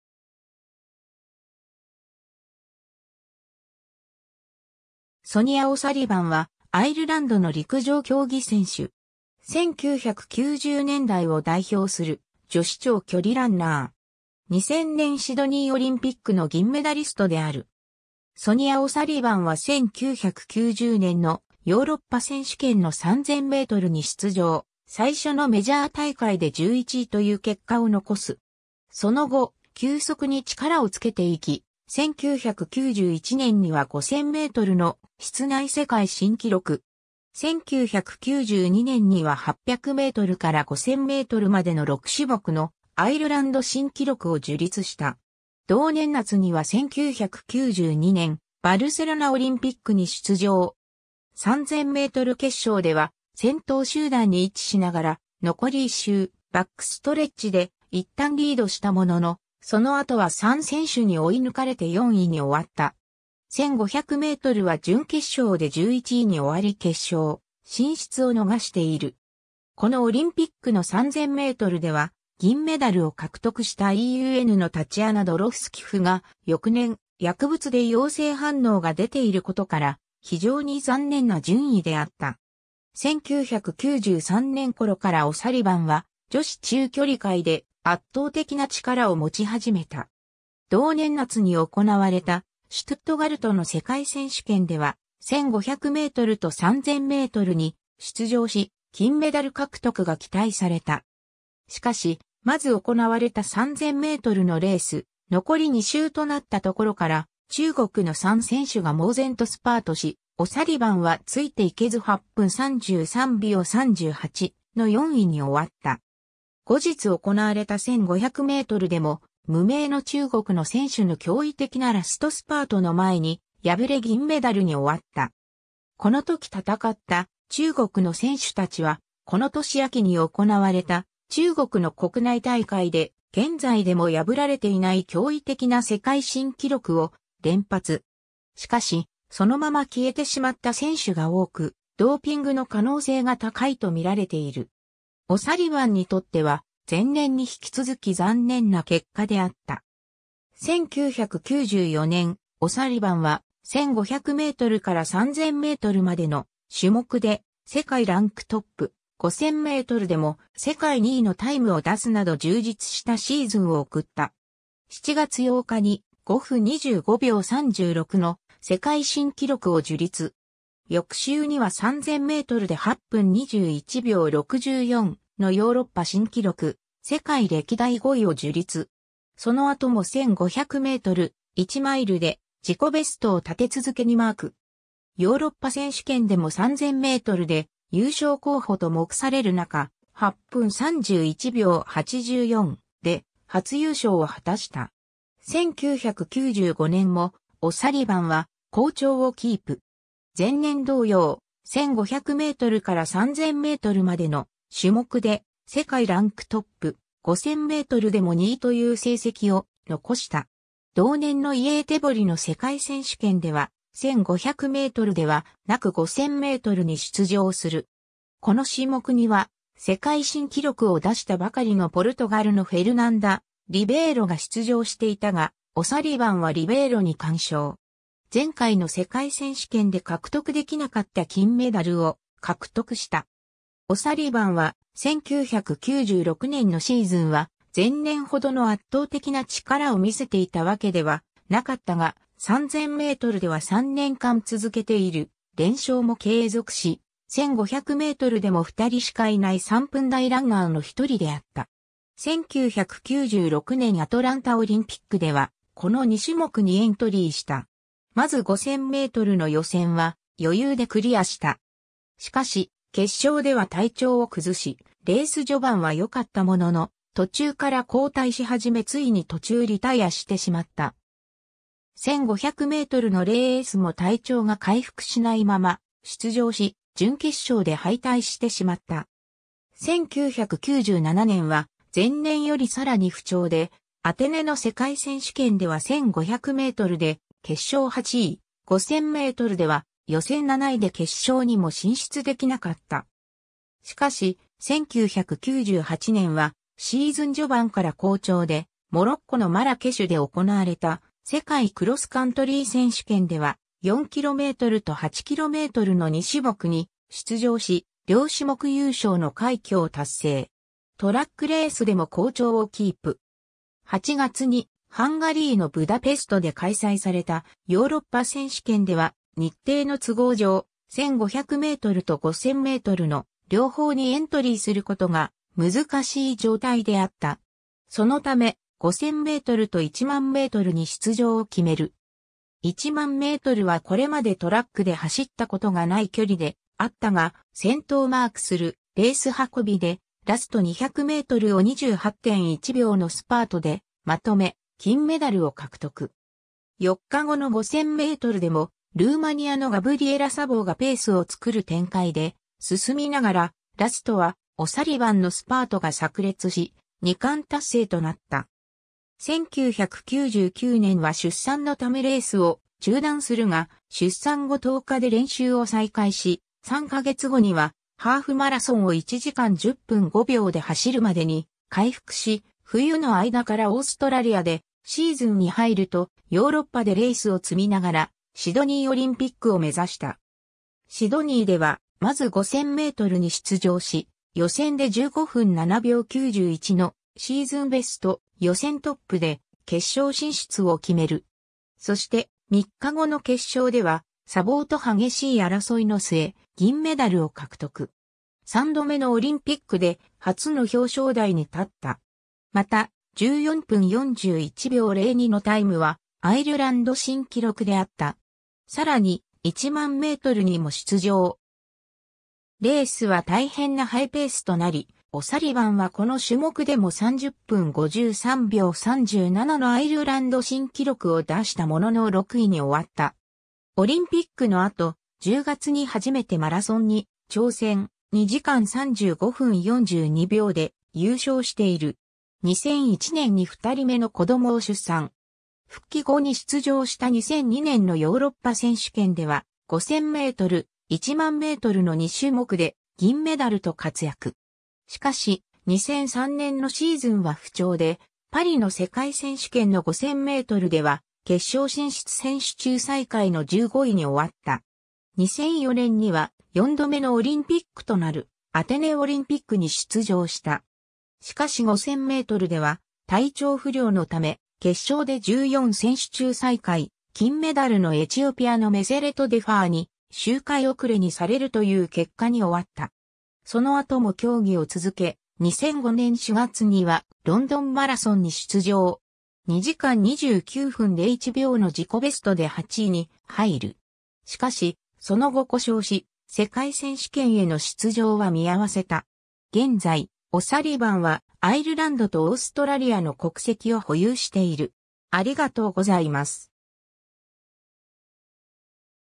ソニア・オサリバン
ソニア・オサリバン（Sonia O'Sullivan、1969年11月28日 - ）は、アイルランドの陸上競技選手。
1990年代を代表する女子長距離ランナー。
2000年シドニーオリンピックの銀メダリストである。
 ソニア・オサリバンは1990年のヨーロッパ選手権の3000mに出場、最初のメジャー大会で11位という結果を残す。
その後、急速に力をつけていき、1991年には5000mの室内世界新記録。
1992年には800mから5000mまでの6種目のアイルランド新記録を樹立した。
 同年夏には1992年バルセロナオリンピックに出場。
3000m決勝では先頭集団に位置しながら残り1周、バックストレッチ（ゴール反対側の直線走路）でいったんリードしたものの、その後は3選手に追い抜かれて4位に終わった。
1500mは準決勝で11位に終わり決勝進出を逃している。
このオリンピックの3000mでは、銀メダルを獲得したEUNのタチアナ・ドロフスキフが、翌年、薬物で陽性反応が出ていることから、非常に残念な順位であった。
 1993年頃からオサリバンは女子中距離界で圧倒的な力を持ち始めた。
同年夏に行われたシュトゥットガルトの世界選手権では、1500mと3000mに出場し、金メダル獲得が期待された。
しかし、まず行われた3000mのレース、残り2周となったところから、中国の3選手が猛然とスパートし、オサリバンはついていけず8分33秒38の4位に終わった。
後日行われた1500mでも、無名の中国の選手の驚異的なラストスパートの前に敗れ銀メダルに終わった。
このとき戦った中国の選手たちは、この年秋に行われた中国の国内大会で、現在でも破られていない驚異的な世界新記録を連発。
しかし、そのまま消えてしまった選手が多く、ドーピングの可能性が高いと見られている。
オサリバンにとっては、前年に引き続き残念な結果であった。
 1994年、オサリバンは、1500mから3000mまでの種目で世界ランクトップ、5000mでも世界2位のタイムを出すなど充実したシーズンを送った。
7月8日に、5分25秒36の世界新記録を樹立。
翌週には3000mで8分21秒64のヨーロッパ新記録、世界歴代5位を樹立（上位4位は前年活躍した中国の選手たちである）。
その後も1500m、1マイルで自己ベストを立て続けにマーク。
ヨーロッパ選手権でも3000mで優勝候補と目される中、8分31秒84で初優勝を果たした。
 1995年も、オサリバンは好調をキープ。
前年同様、1500mから3000mまでの種目で世界ランクトップ、5000mでも2位という成績を残した。
同年のイェーテボリの世界選手権では、1500mではなく5000mに出場する。
この種目には、世界新記録を出したばかりのポルトガルのフェルナンダ・リベイロが出場していたが、オサリバンはリベイロに完勝。
前回の世界選手権で獲得できなかった金メダルを獲得した。
 オサリバンは、1996年のシーズンは前年ほどの圧倒的な力を見せていたわけではなかったが、3000mでは3年間続けている連勝も継続し、1500mでも2人しかいない3分台ランナーの1人であった。
1996年アトランタオリンピックではこの2種目にエントリーした。
まず5000mの予選は余裕でクリアした。
しかし、決勝では体調を崩し、レース序盤はよかったものの、途中から後退しはじめついに途中リタイアしてしまった。
1500mのレースも体調が回復しないまま出場し、準決勝で敗退してしまった。
 1997年は、前年よりさらに不調で、アテネの世界選手権では1500mで決勝8位、5000mでは予選7位で決勝にも進出できなかった。
しかし、1998年はシーズン序盤から好調で、モロッコのマラケシュで行われた世界クロスカントリー選手権では、4kmと8kmの2種目に出場し、両種目優勝の快挙を達成。
トラックレースでも好調をキープ。
8月にハンガリーのブダペストで開催されたヨーロッパ選手権では、日程の都合上、1500mと5000mの両方にエントリー...